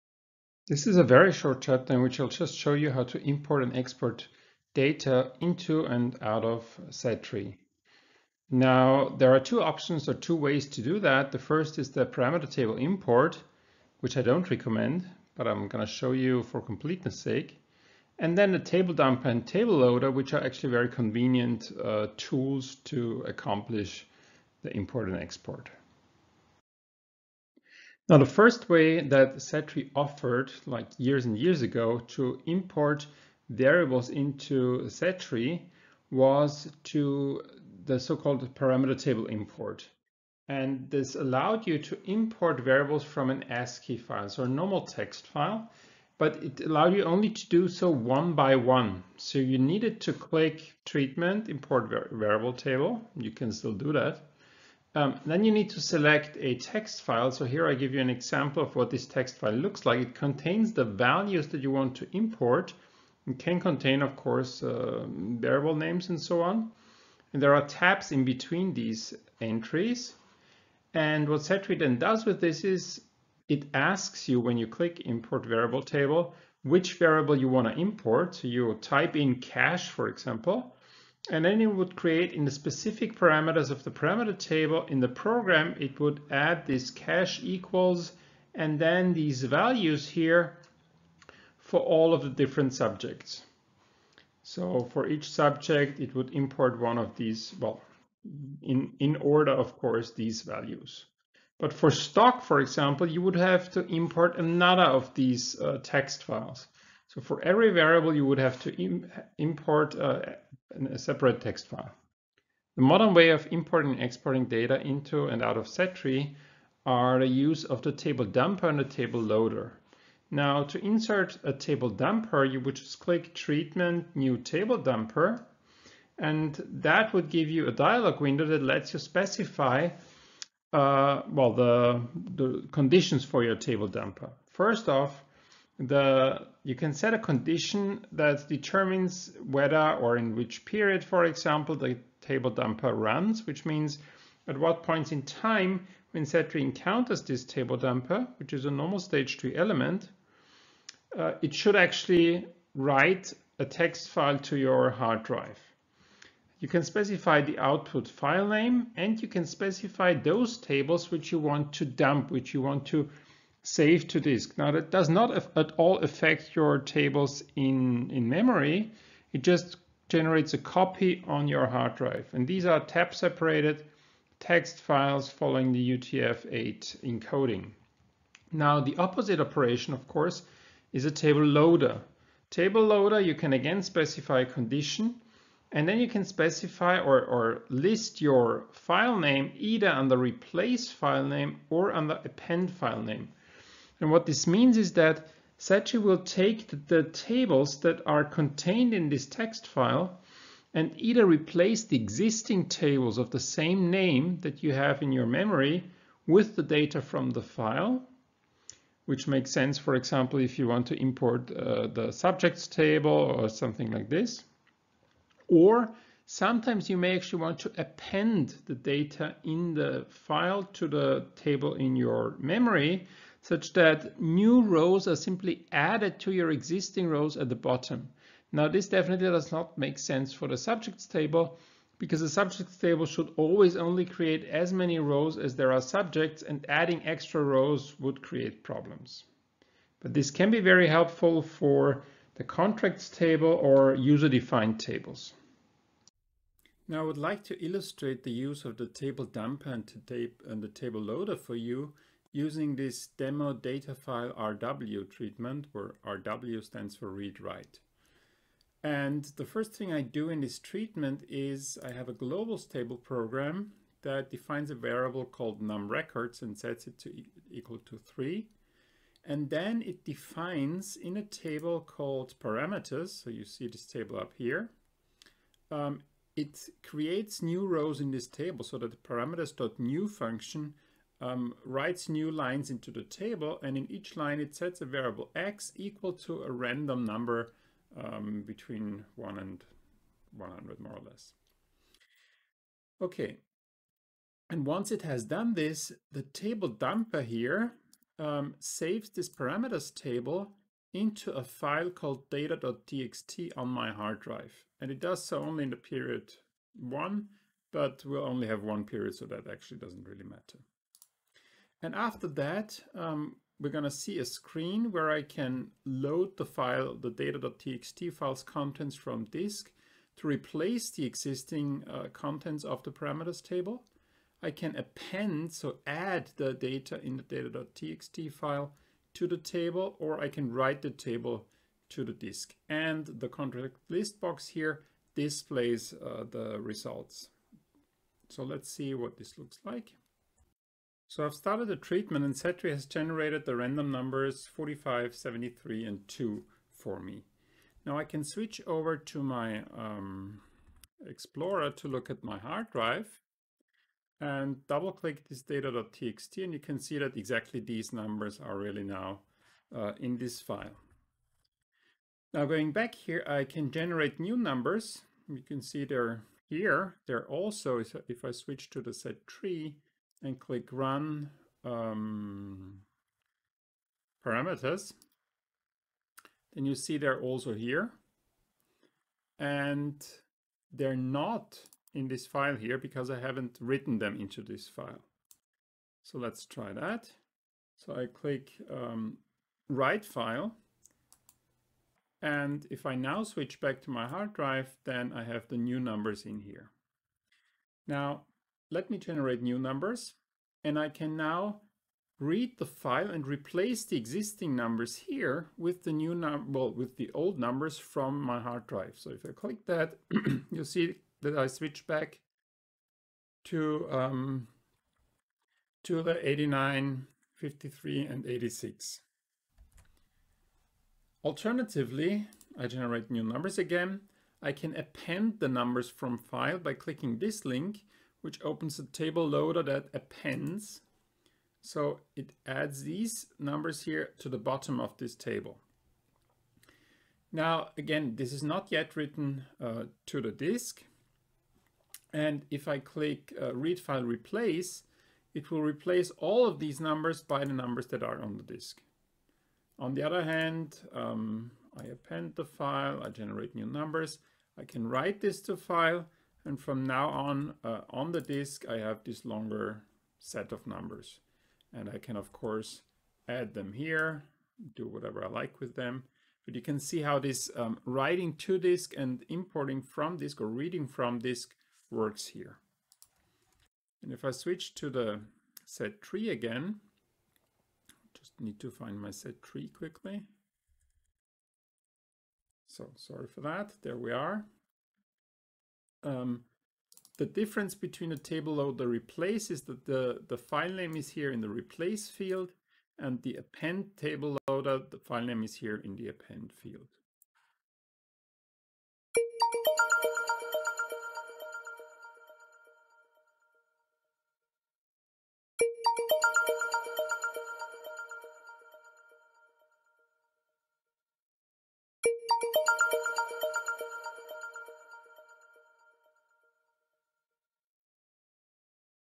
Import and export | z-Tree online course
This chapter discusses how to best import z-Tree results data into analysis software such as Excel, R, Stata and others.

0:00 Agenda
0:26 Parameter table import
1:52 Table dumper/table loader
4:06 Data import and export demo
10:21 End screen

for all course material.